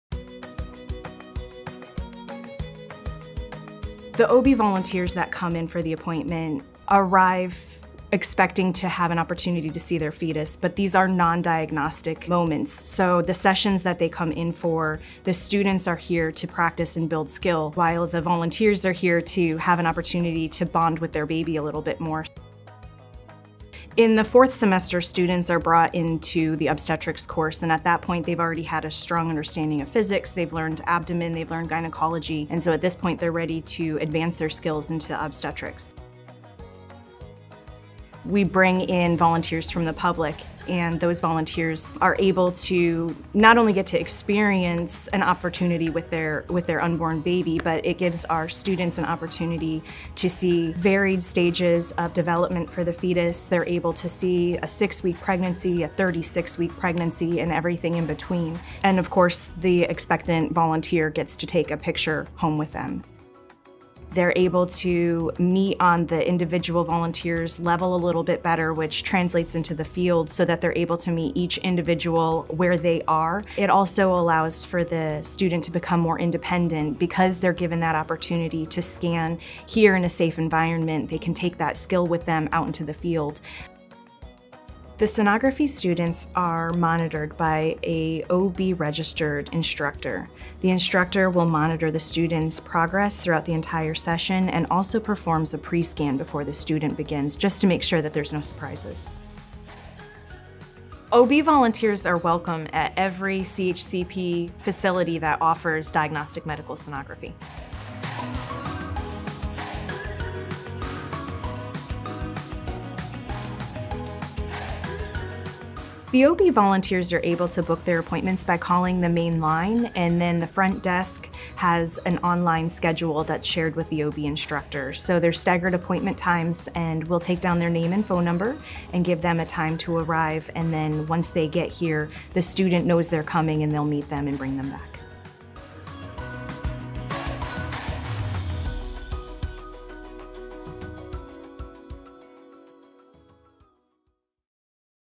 Ultrasound Volunteer Program | CHCP
CHCP provides non-diagnostic ultrasounds to expectant mothers. Ultrasound sessions are performed by Diagnostic Medical Sonography students and are intended for training purposes only.

⚠️OB Volunteer Requirements: ⚠️
- Must have had a prior diagnostic ultrasound of their current pregnancy by their primary provider.
- Must sign a volunteer program waiver.

The College of Health Care Professions is a Texas-based career education school dedicated to providing students with the best health care career training.